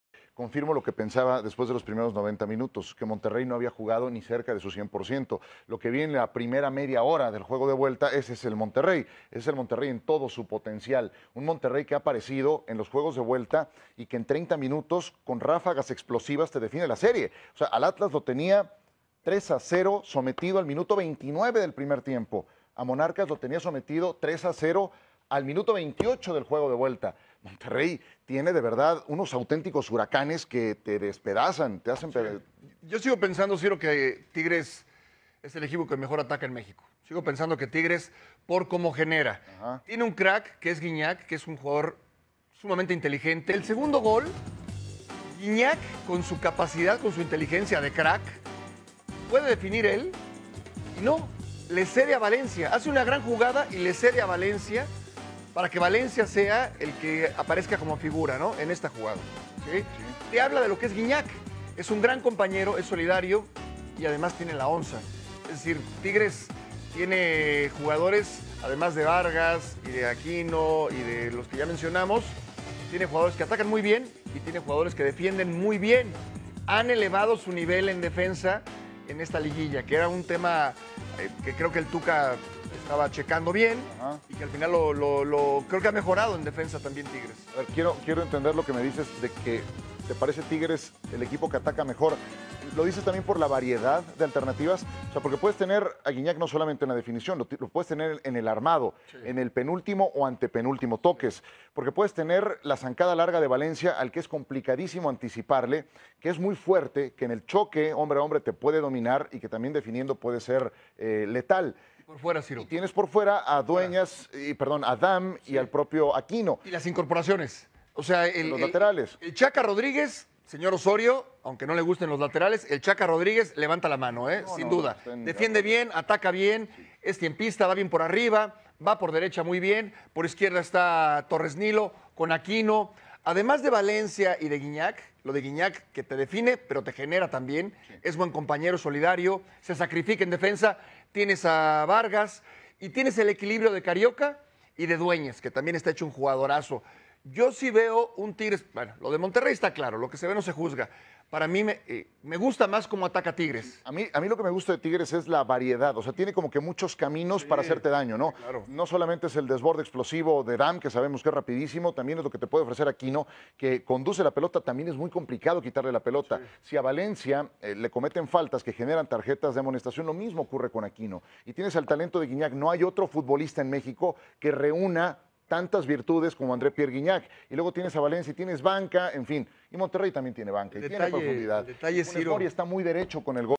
¿Tiene TIGRES el mejor ataque de la LIGA MX?
Paco Gabriel y Ciro analizan las opciones que tienen al frente el equipo universitario y Rayados.